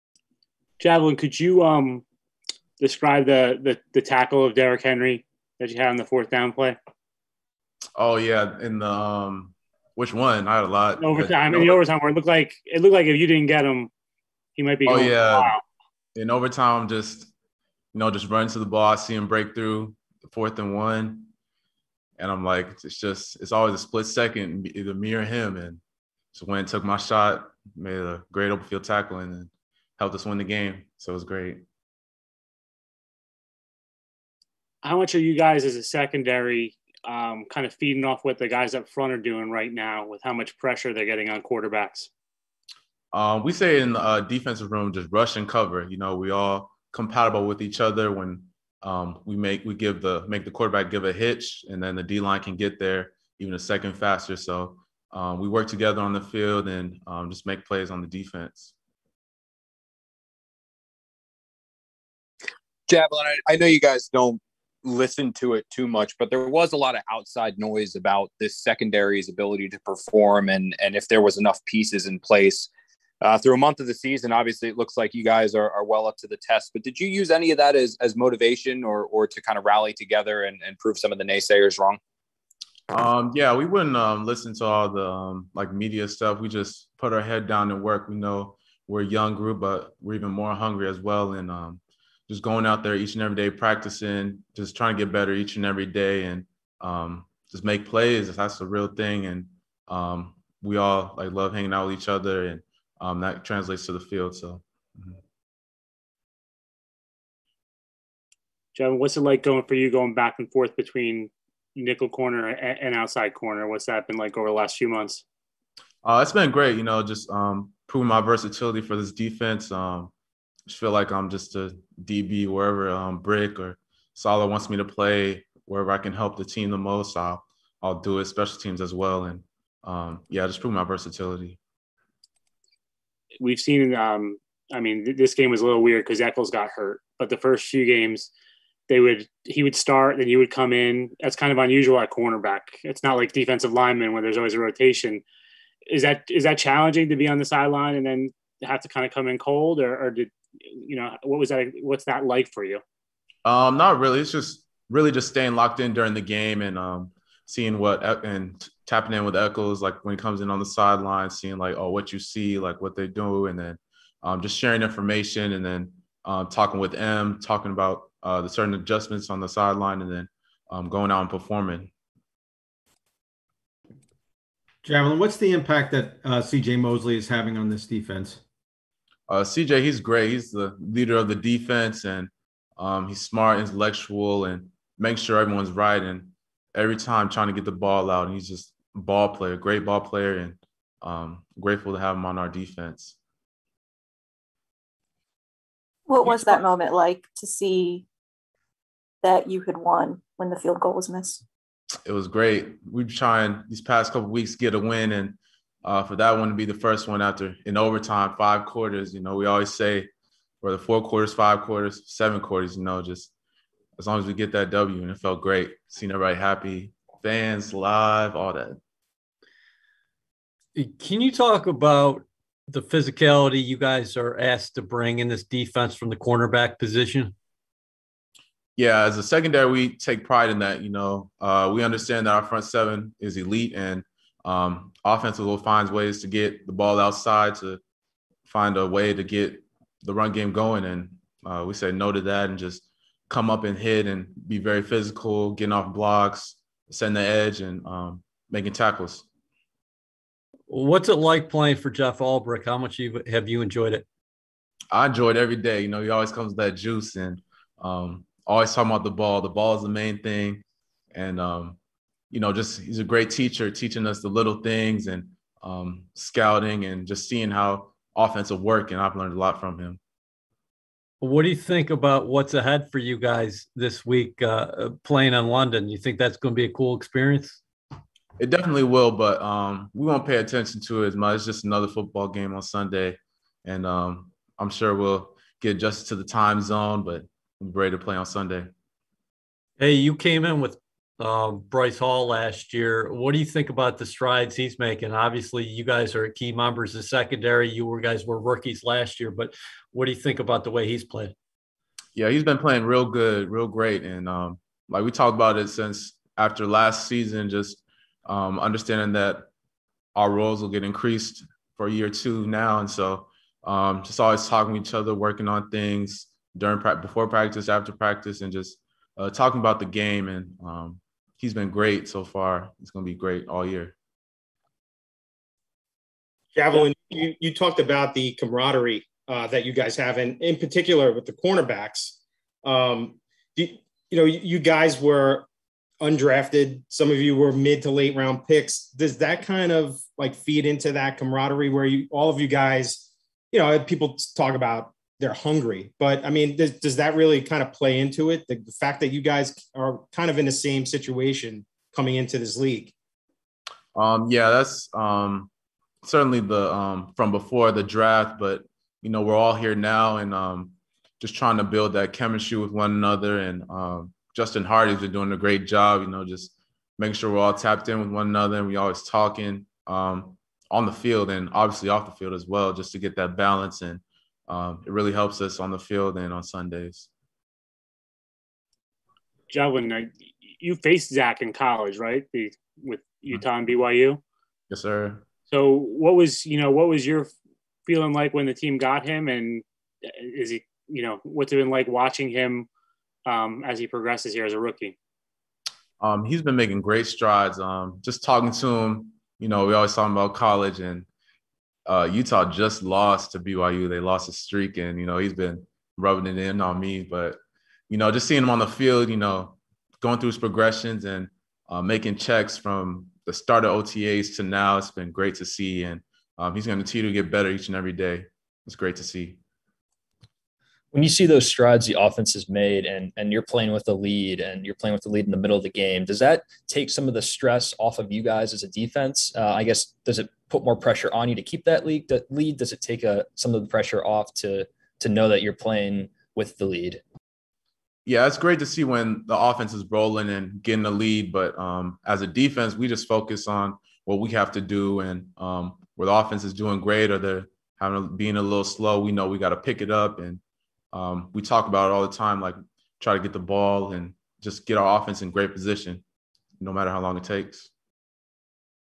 "We Put Our Head Down & Work" | Javelin Guidry Media Availability | The New York Jets | NFL
CB Javelin Guidry speaks to the media Monday, October 4 after the team's overtime victory against the Tennessee Titans.